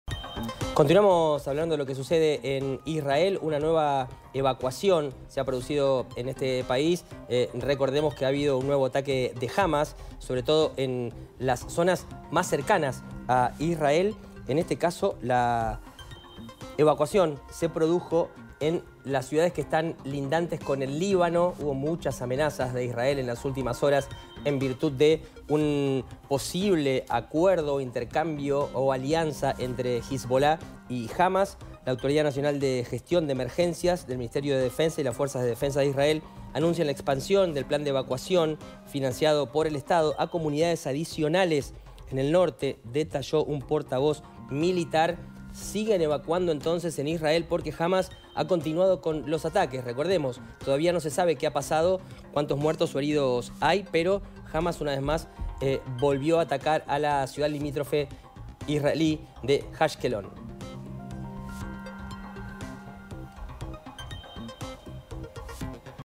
Israel evacua otras 14 comunidades del norte ante aumento de tensión con Hizbulá
👉 Israel ordenó la evacuación de otras catorce comunidades del norte, próximas a la frontera con Líbano, mientras intensifica sus ataques contra infraestructura militar de la milicia chií Hizbulá.